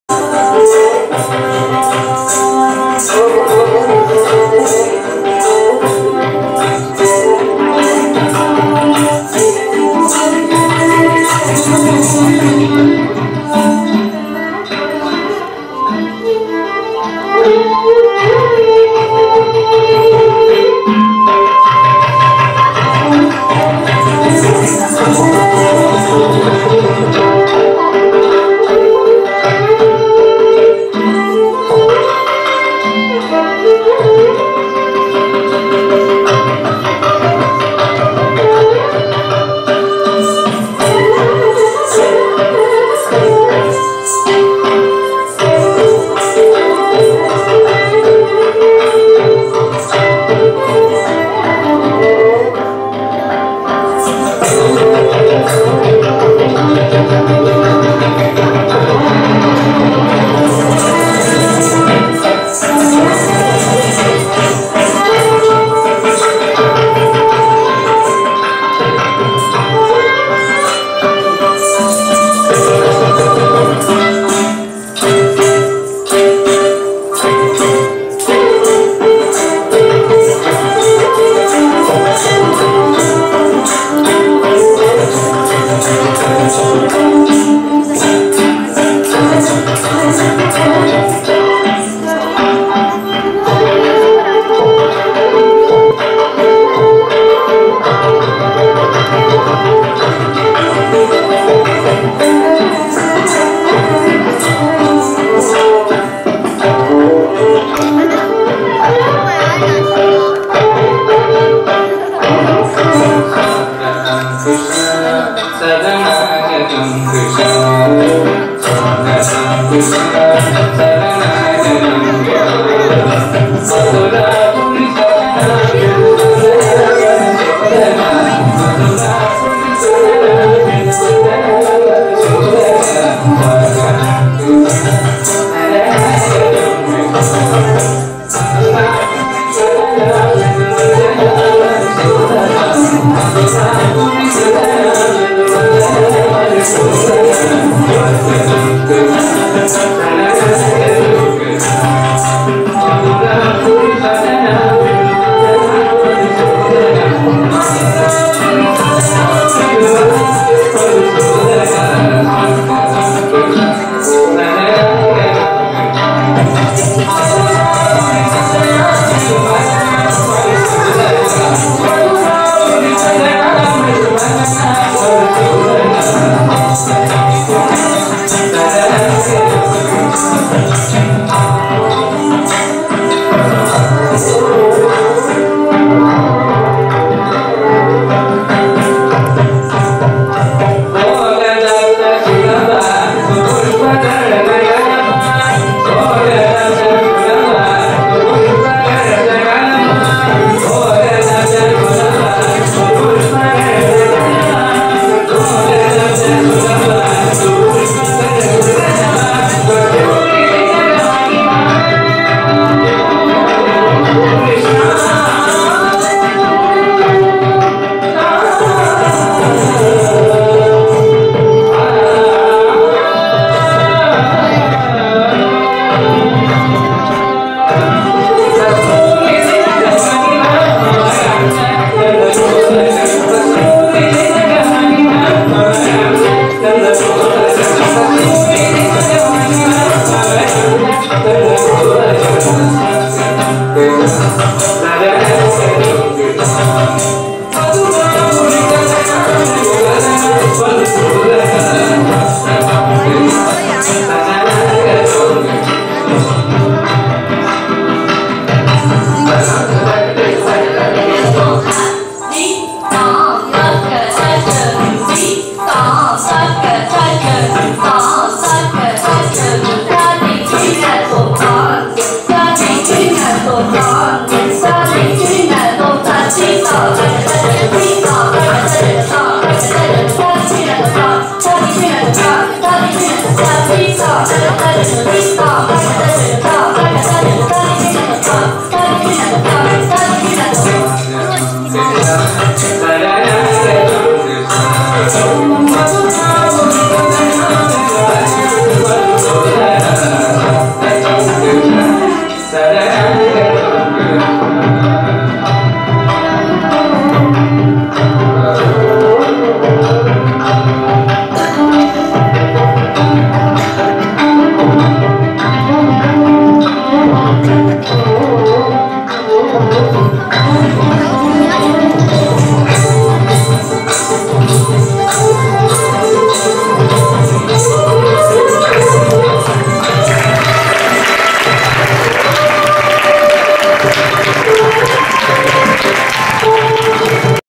Sai Baba 89 Bday Part 4 23 11 2014
Markham Sai Center Celebrated Sri Sathya Sai Baba's 89th Birthday